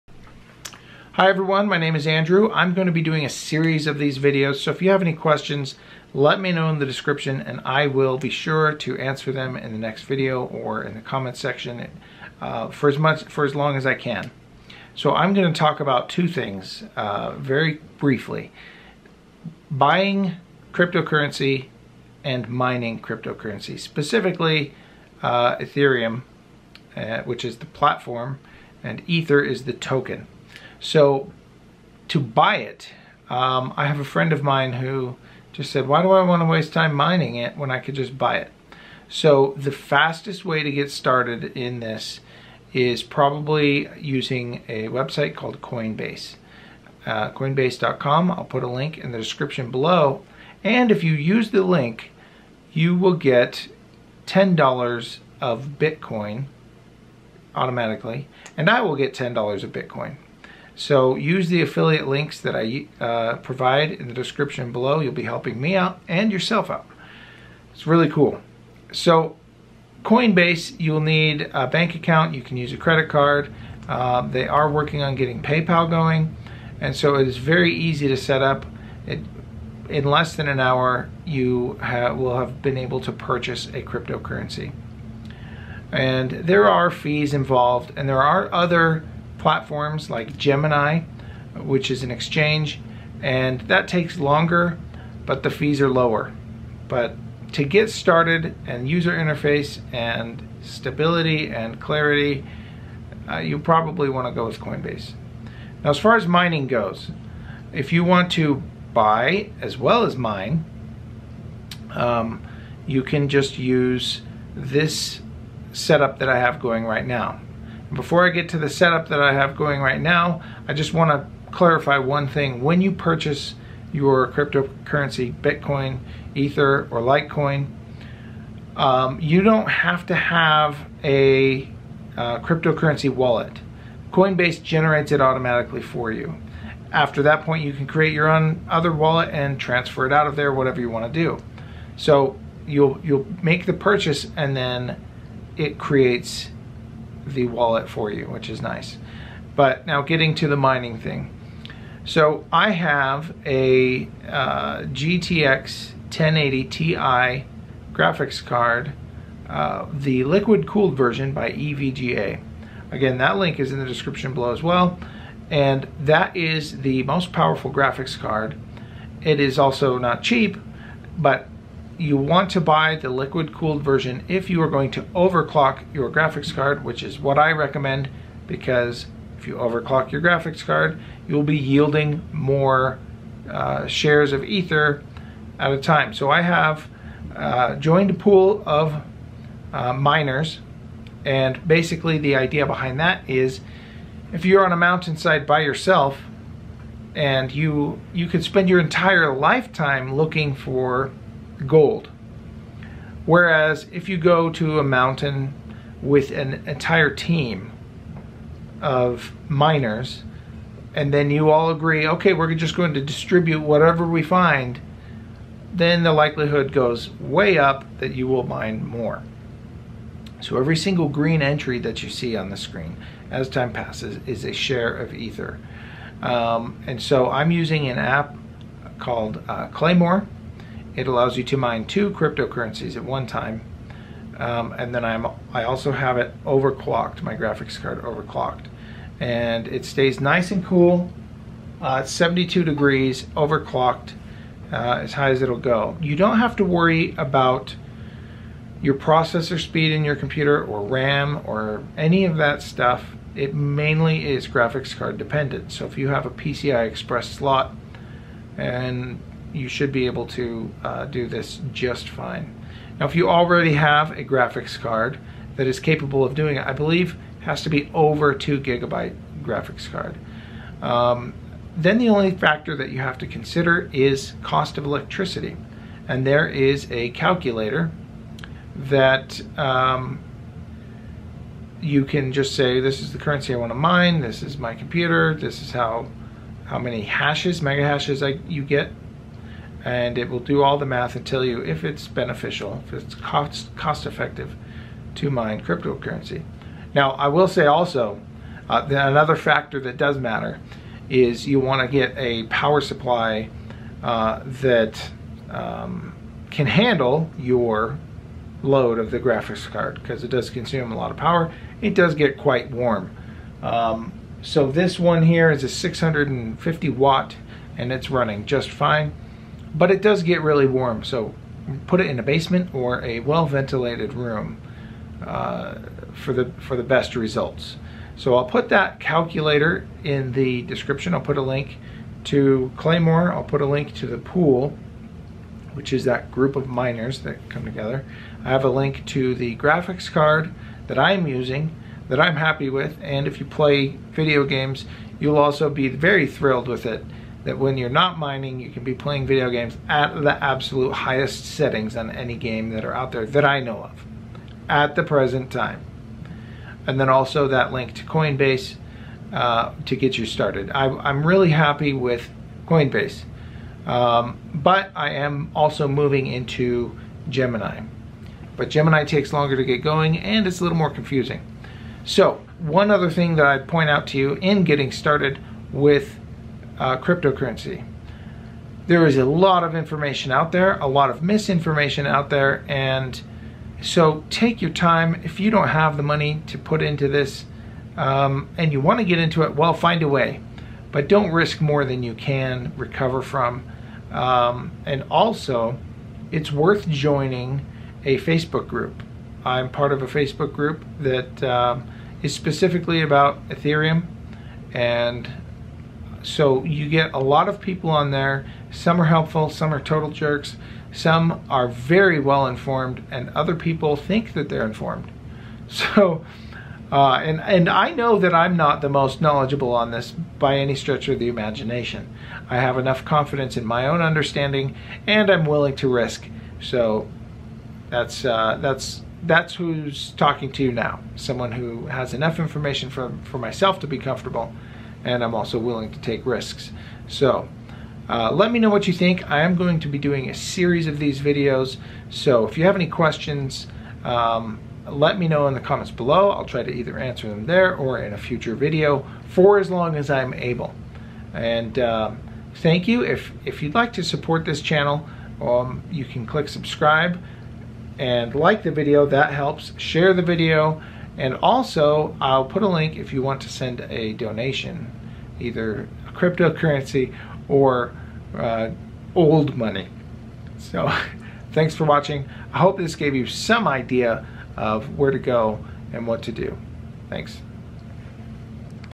Buying & Mining Ethereum Cryptocurrency Basics
There’s a lot to figure out when getting started.  Here’s some basic points.  I’ll be covering the technical step by step instructions of mining, buying, selling etc… in separate future videos.

Graphics Card
Power Supply

THANKS! It all helps me in keeping these videos coming!